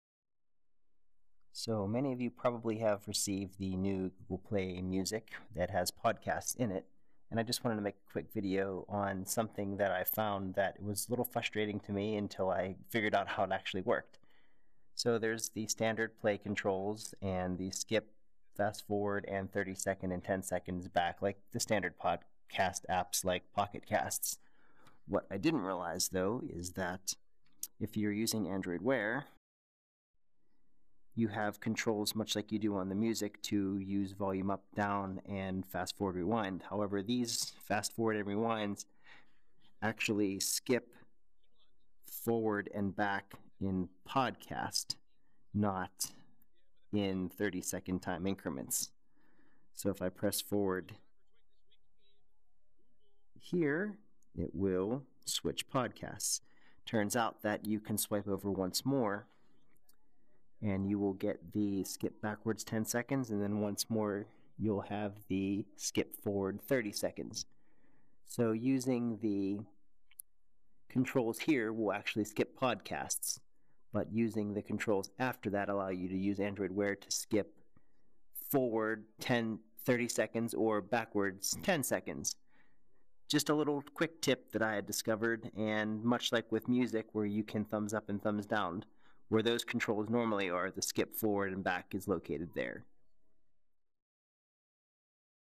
Quick tip: Google Play Music With Podcasts and Android Wear
A quick tip for controlling google play music podcasts on android wear. I didn't realize initially where the controls were on android wear to skip ahead 30 seconds and back 10 seconds.  Hopefully this will help others who also didn't see it at first!